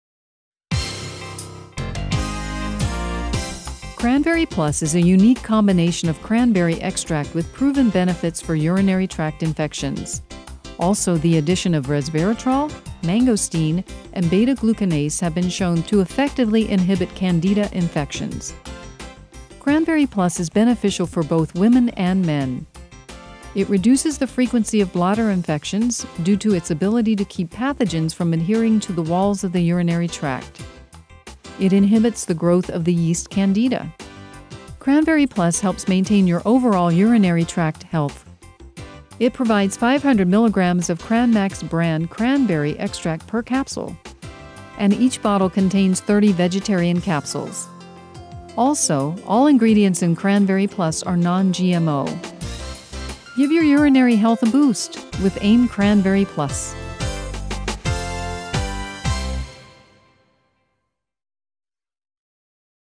CranVerry+ CranVerry Plus
CranVerry+
30 vegetarian capsules

CranVerry+ harnesses the awesome, protective powers of cranberries. Each capsule contains 500 mg of cranberry extract, the same as thirty-four pounds of cranberries. It’s formulated for men and women to help maintain urinary tract health and to effectively inhibit Candida growth.

Urinary Health
Inhibit Candida Growth
May reduce the frequency of bladder infections